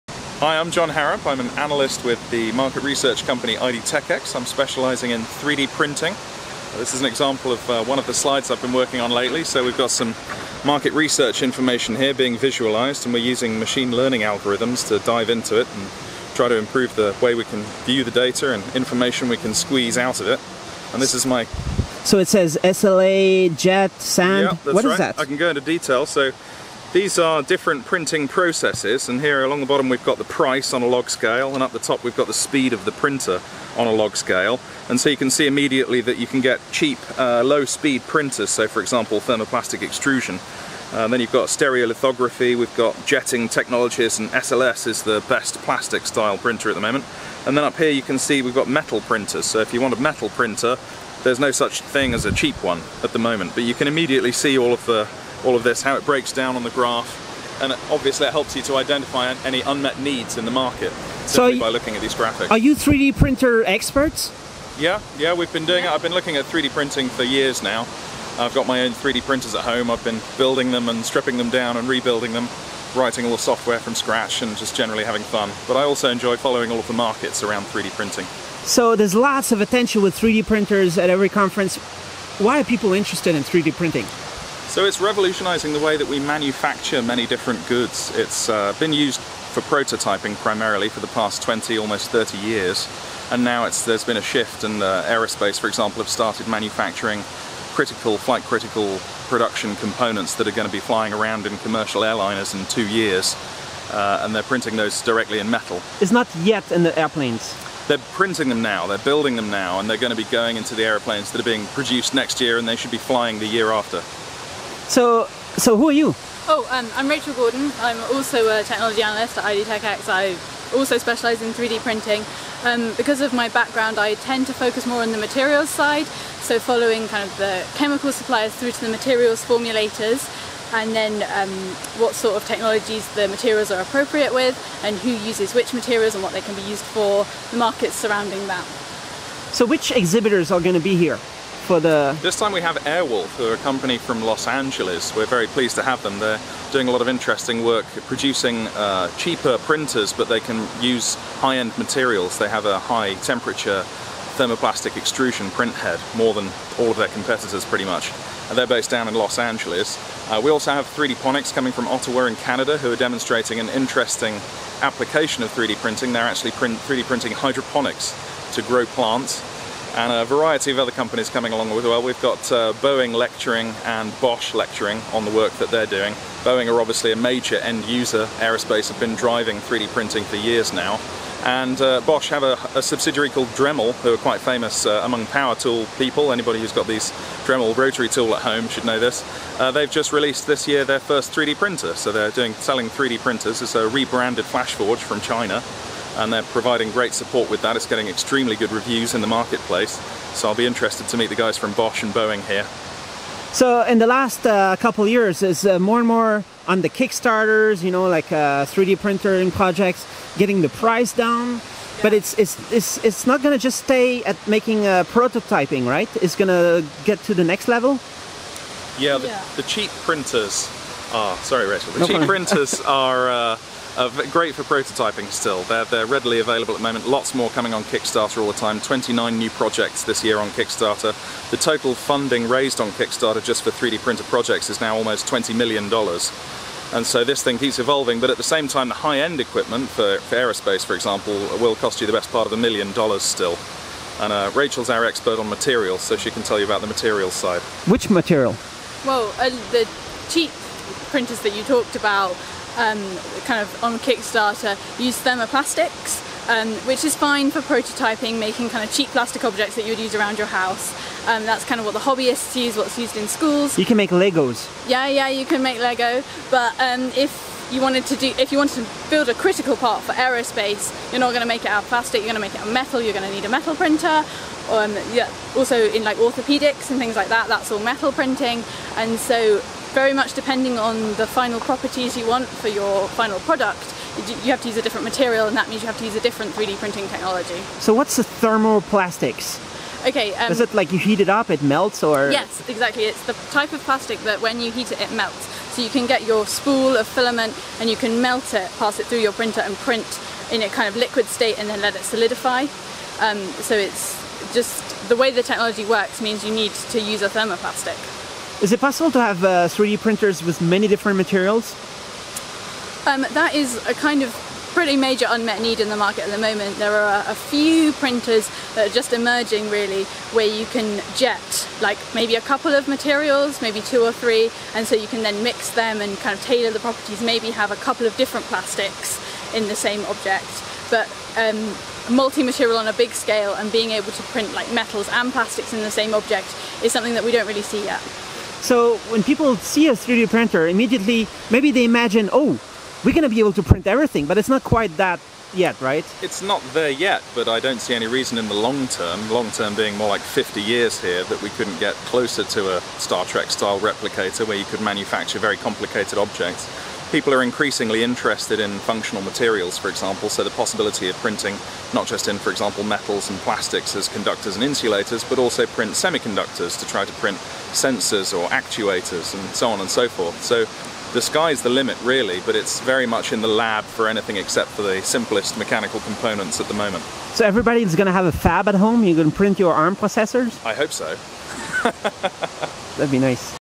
3D Printing, opportunity gap assessment and what’s coming
Dr Jon Harrop and Rachel Gordon, analysts at IDTechEx, explain how unique software developed by IDTechEx shows the range of performance of different 3D printing technologies from 208 3D printer suppliers. This identifies unmet needs/gaps in the 3D printer market, by technology type and other parameters. There is also huge innovation in materials used for 3D printing – and still big needs. IDTechEx have also found that 3D printer projects on Kickstarter have raised almost $20million to date.